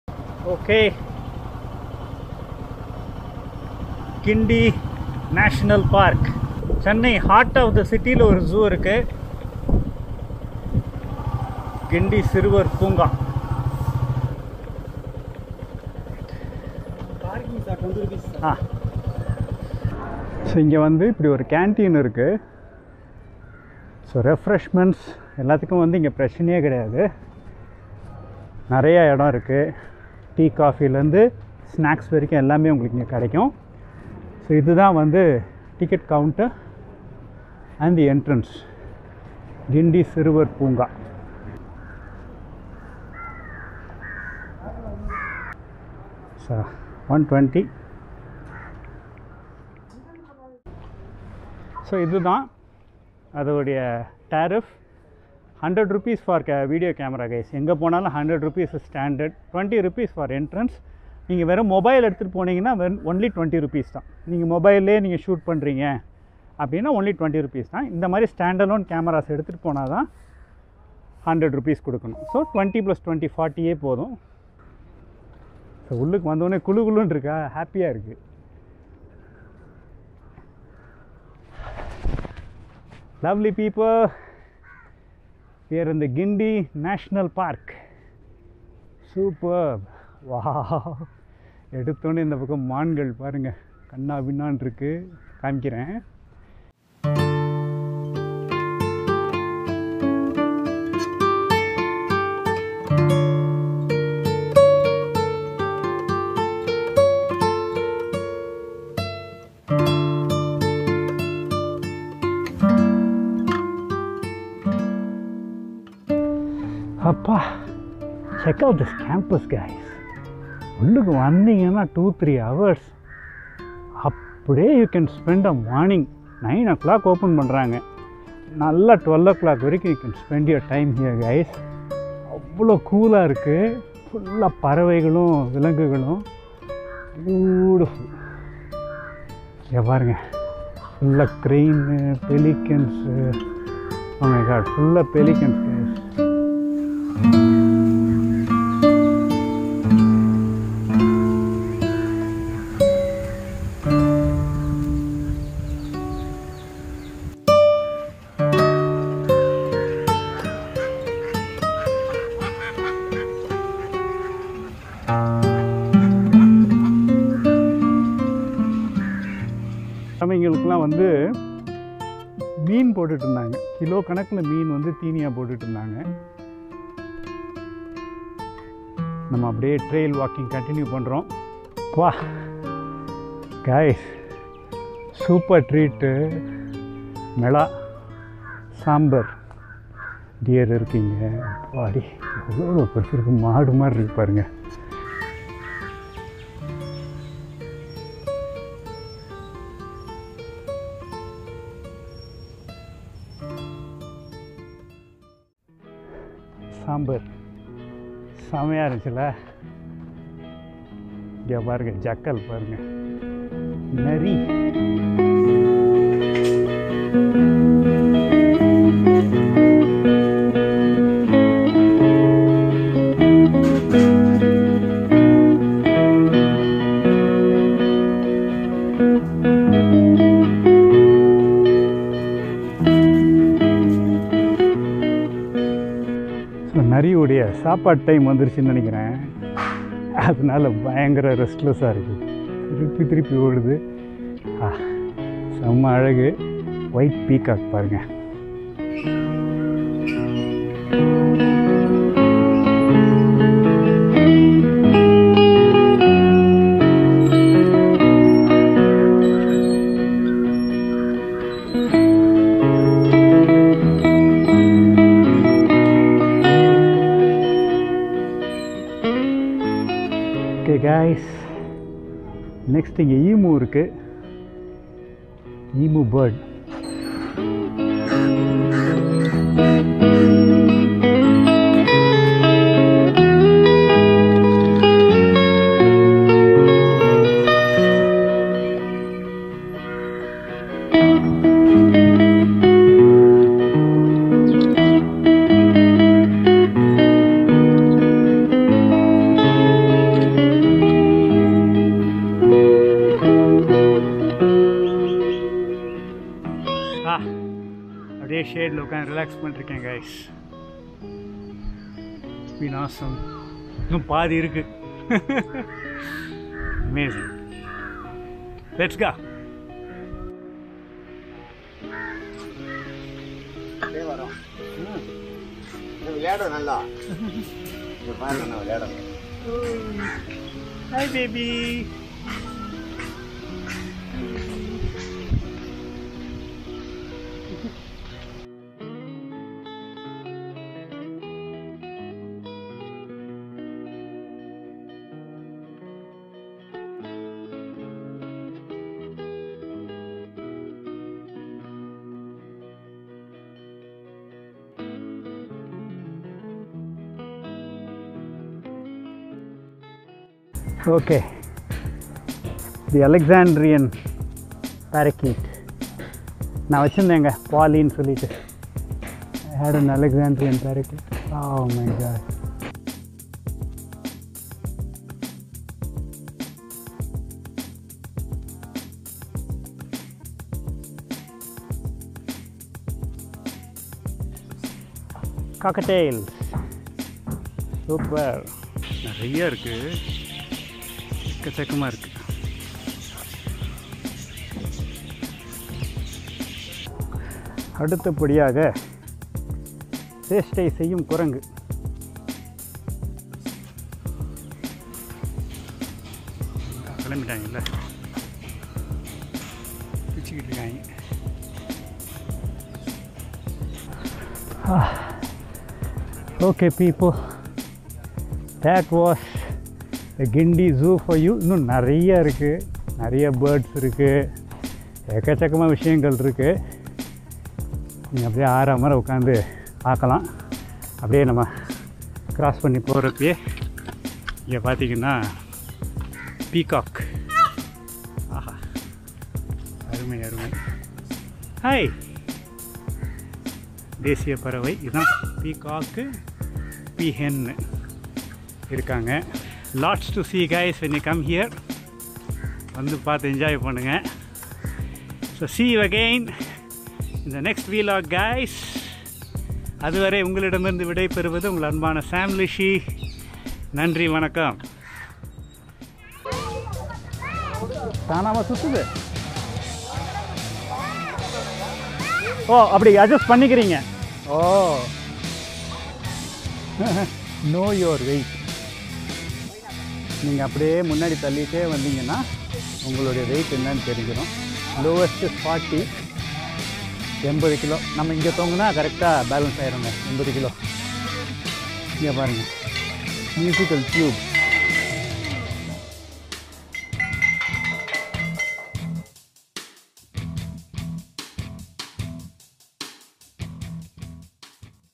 Guindy national park | zoo in the heart of Chennai | மெட்ராச சுத்தி பாக்க போறோம் ep_5
*Zoo in the heart of Chennai City*
மெட்ராஸை சுத்தி பார்க்க போறோம் ep_5

The Guindy National park is located right in the heart of Chennai city. It truly is a treat as you can access the zoo within 30 minutes from anywhere in the City. The zoo is a combination of both a wide variety of animals as well as acres of shady trees and a kids play area for your family to enjoy. It's a perfect place for you to go and spend a couple of hours with beautiful animals and nature alike!!